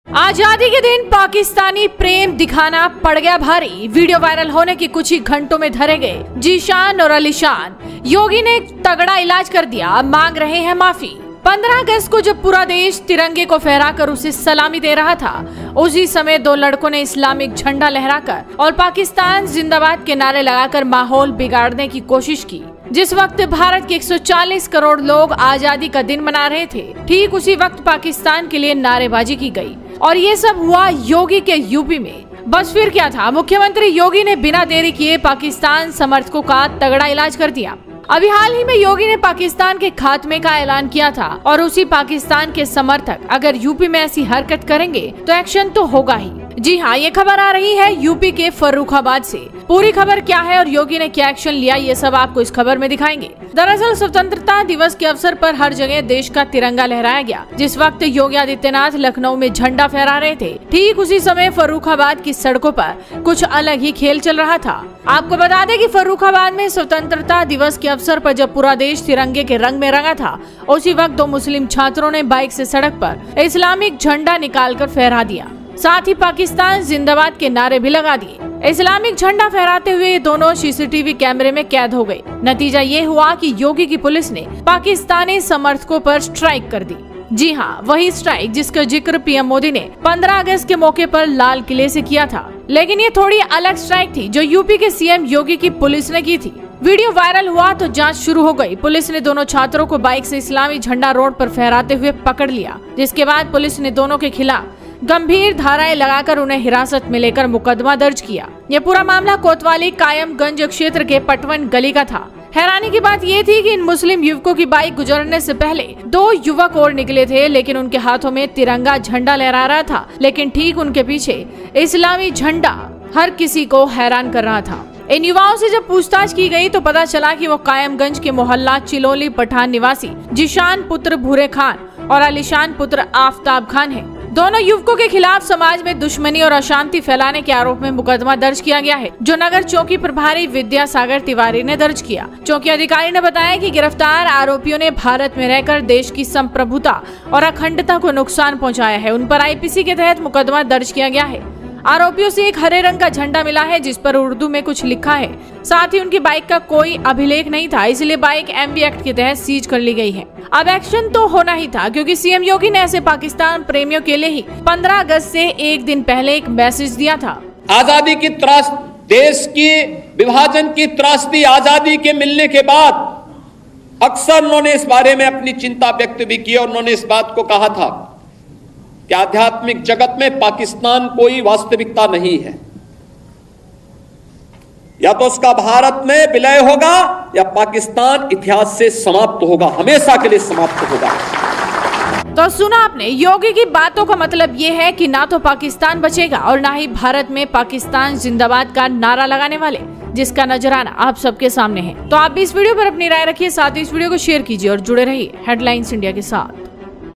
आजादी के दिन पाकिस्तानी प्रेम दिखाना पड़ गया भारी, योगी ने तगड़ा इलाज कर दिया । Headlines India
आजादी के दिन पाकिस्तानी प्रेम दिखाना पड़ गया भारी, योगी ने तगड़ा इलाज कर दिया